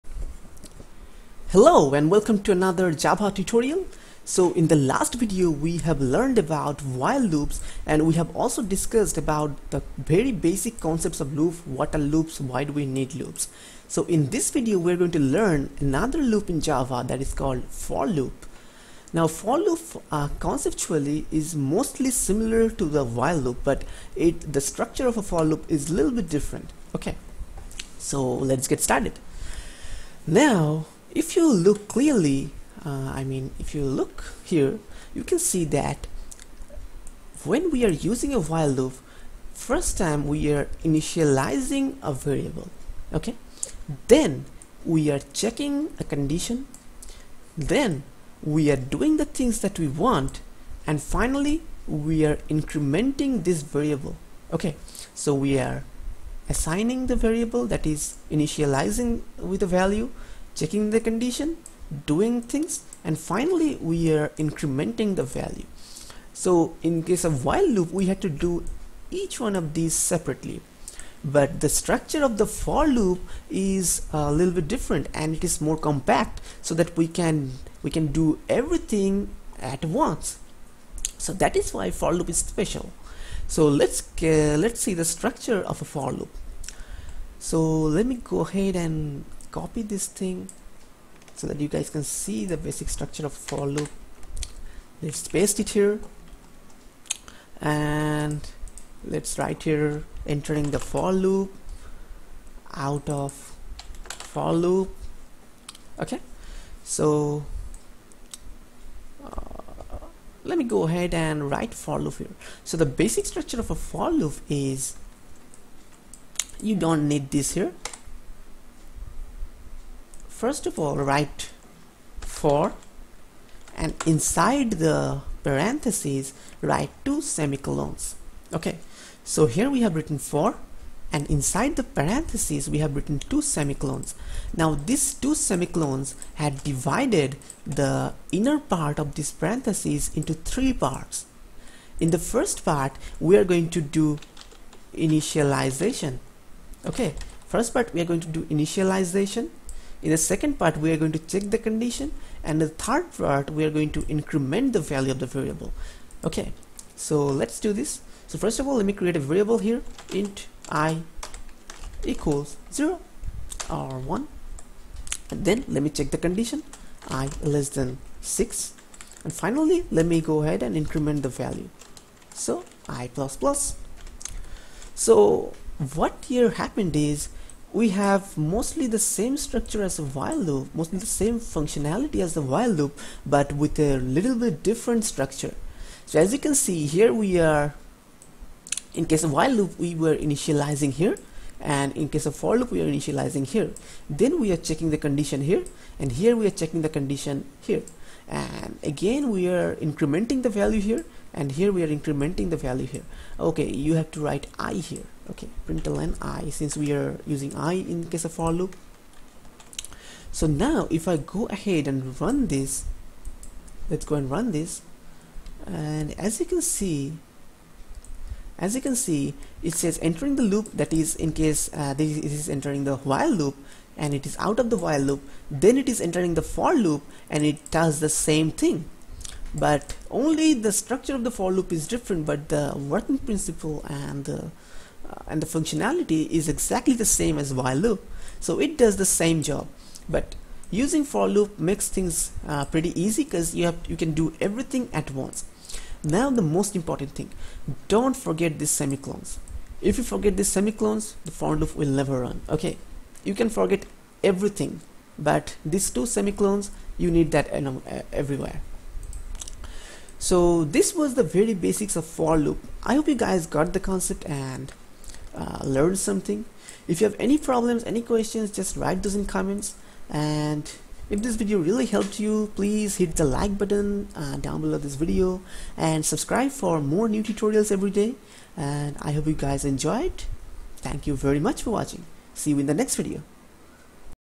Java Tutorial For Beginners- 13 - For loops in Java
Video Description:
In this video we will learn about For loops in java and we'll also learn about the basic concepts of a loop.

If you have any questions, make sure to write those in comments.

Thank you very much, for watching my video, Have a great day.

It will help me to continue doing free tutorials, god bless you. :)